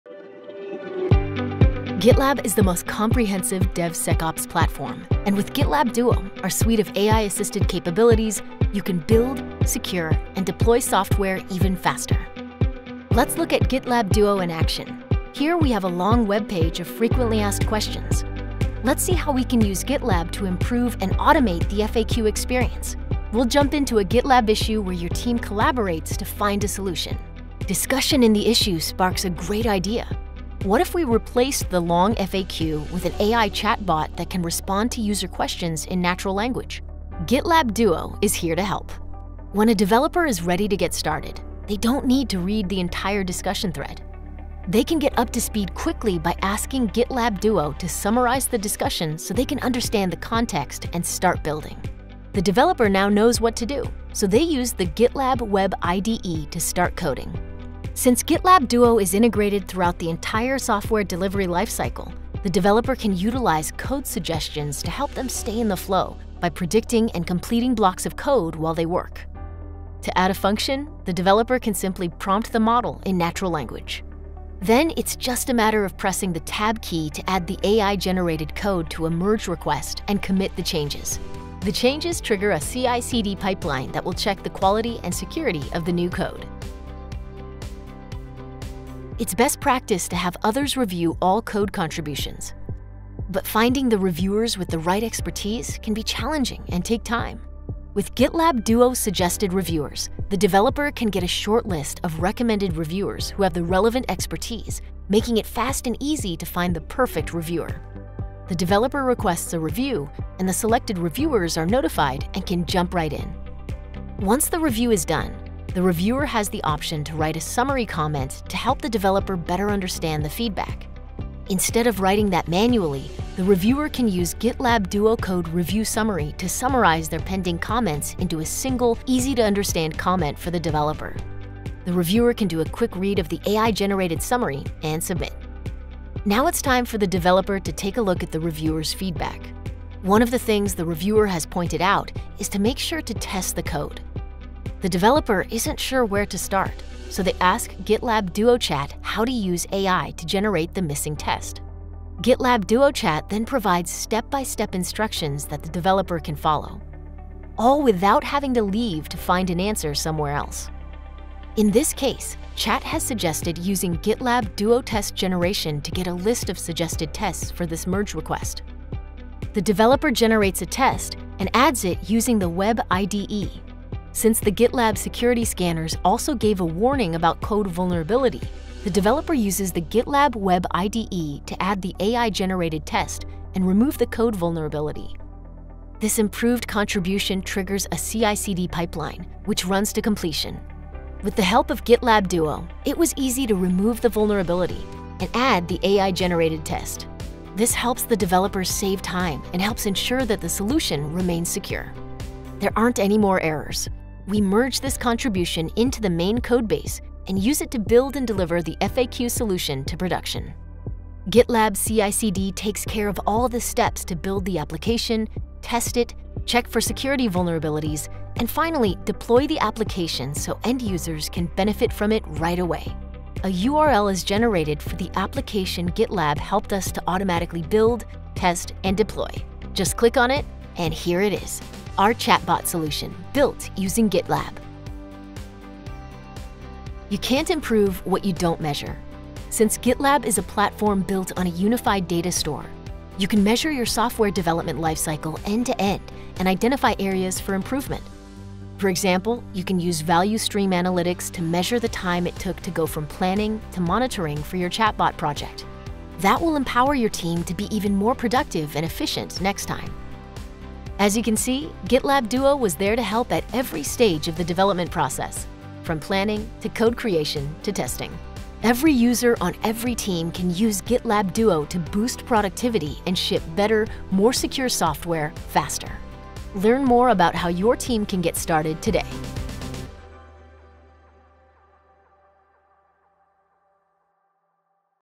GitLab Duo in Action
Build, test and deploy a chatbot with the help of GitLab Duo. Boost efficiency and reduce cycle times with the help of AI at every step of the software development lifecycle.